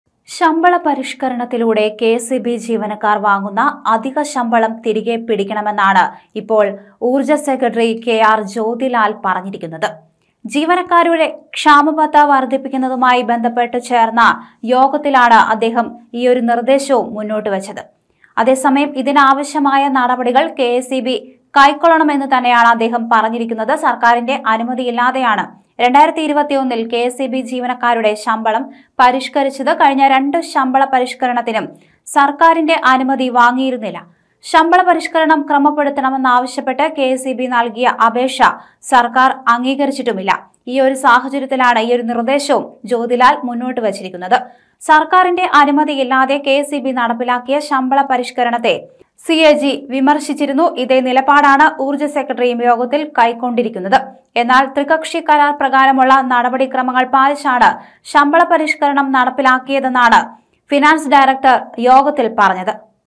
കെഎസ്ഇബിയിലെ അധിക ശമ്പളം തിരിച്ചുപിടിക്കാന്‍ നീക്കം തകൃതി I KSEB
കെഎസ്ഇബി ജീവനക്കാര്‍ വാങ്ങുന്ന അധികശമ്പളം തിരികെ പിടിക്കണമെന്നു ഊര്‍ജ സെക്രട്ടറി കെ.ആര്‍.ജ്യോതിലാല്‍.ഇതിനാവശ്യമായ നടപടികള്‍ കെഎസ്ഇബി കൈക്കൊള്ളണമെന്ന് അദ്ദേഹം പറഞ്ഞു.സര്‍ക്കാരിന്റെ അനുമതിയില്ലാതെയാണ് 2021ല്‍ കെഎസ്ഇബി ജീവനക്കാരുടെ ശമ്പളം പരിഷ്‌കരിച്ചത്